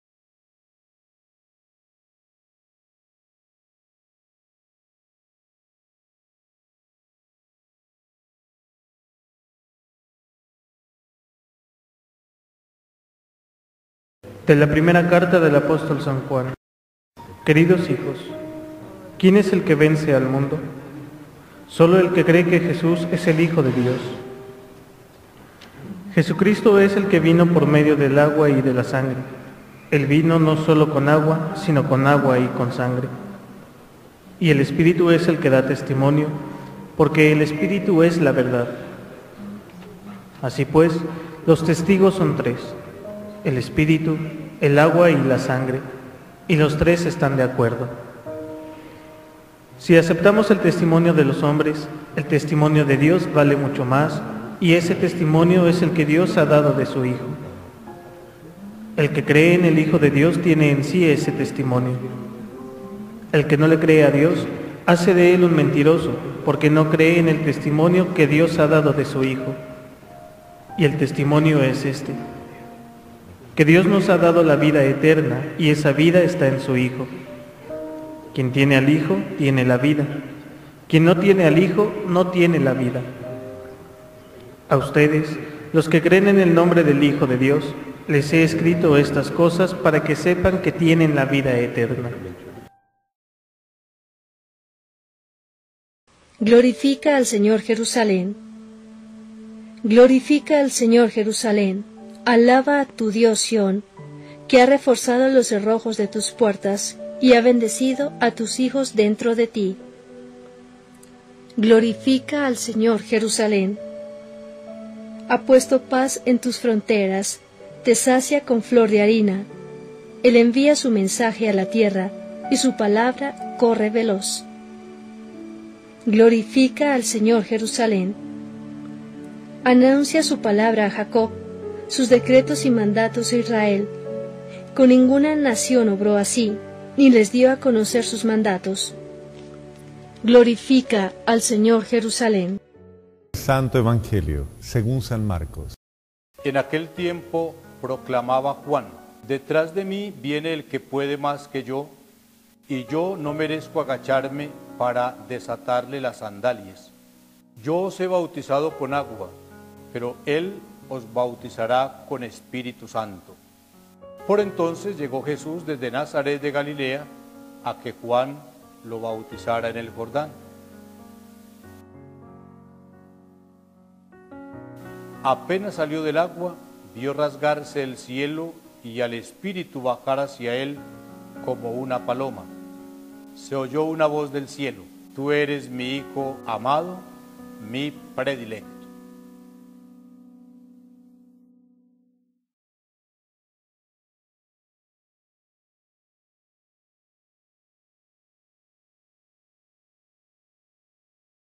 Evangelio y Lecturas Sábado donde la Epífania se celebra el domingo siguiente al 1 de enero
[Donde la Epifanía se celebra el Domingo siguiente al 1 de Enero] Tiempo de Navidad Primera Lectura: Epístola I de Juan 5, 5-13 Salmo: 147 R/.Glorifica al Señor, Jerusalén Evangelio según San Marcos 1, 7-11 EVANGELIO Y LECTURAS TIEMPO DE NAVIDAD